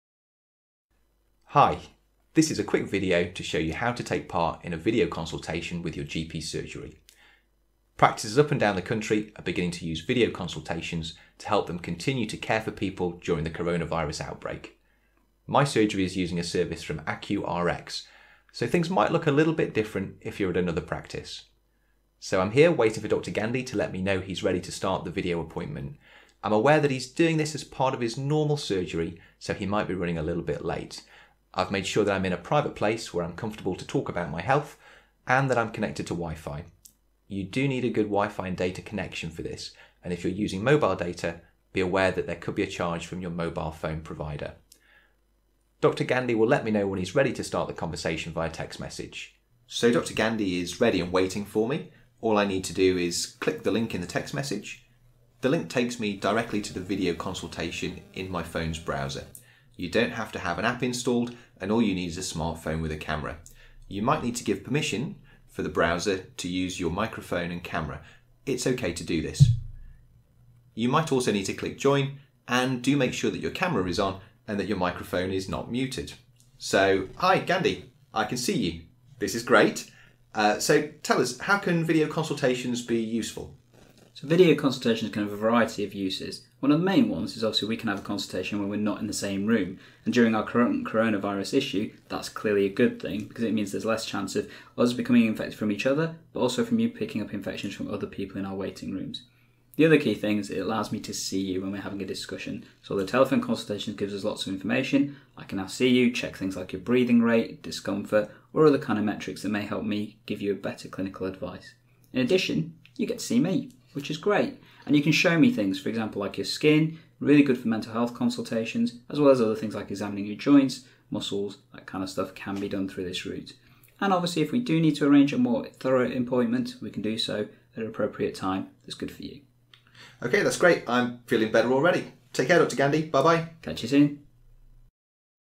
Have a video consultation with your doctor
A short video to show you how to have a video consultation with your GP surgery after receiving an invitation via text message.

Increased use of telephone and video consultations is expected during the current COVID19 situation.

We are using the AccuRX system in the video. The system and process used by other practices may vary.

A link to this video can be sent to a patient's phone by the surgery when a video appointment is booked so that they can prepare themselves for their video consultation.

We are happy for practices, PCNs or anyone to use and distribute this video.